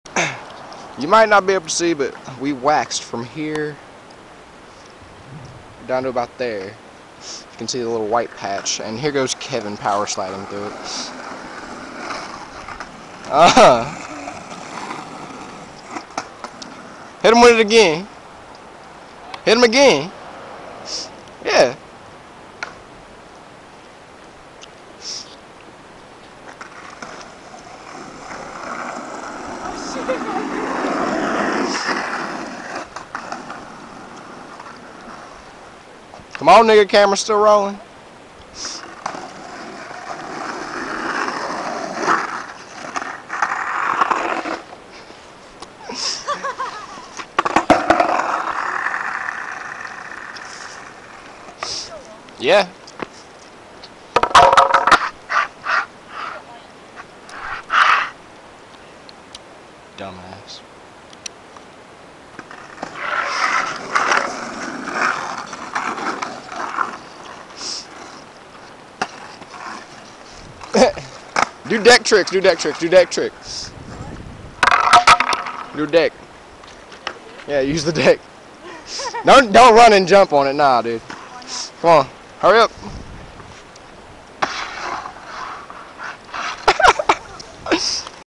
Kevin Slides
...me and kevin had an idea...wax my decks and wax the road...so i tape him powersliding thru it on my board and then on a deck lol